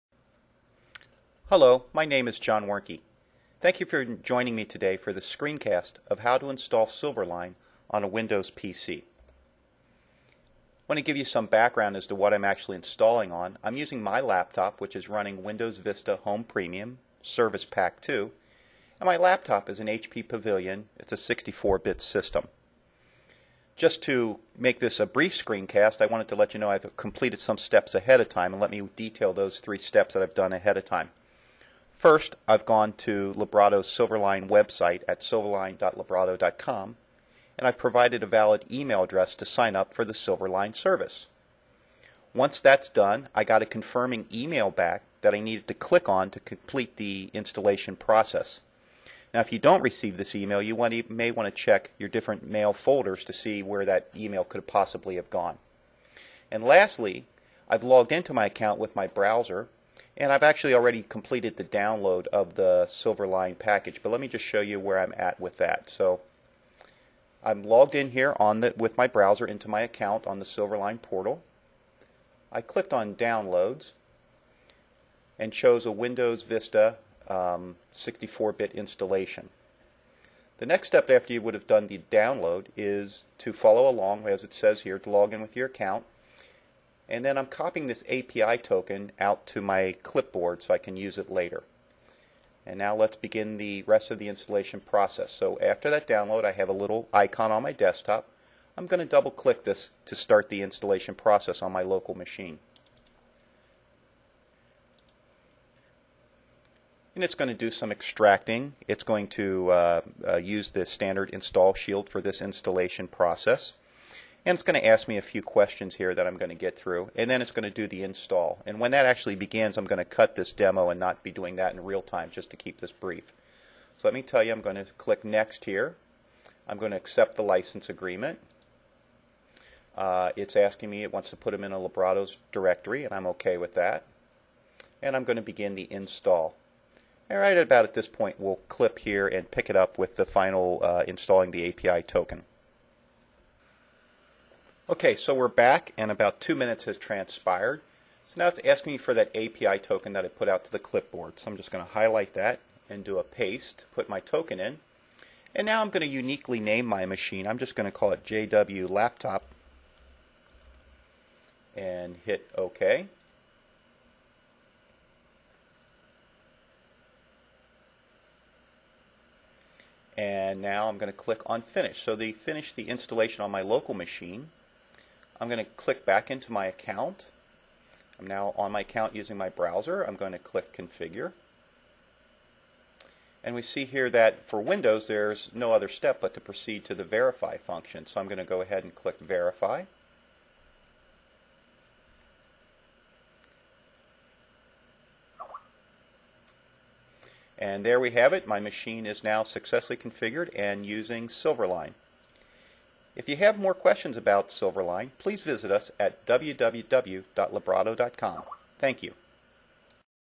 Silverline Window Installation
Screencast of installing SIlverline on a Windows system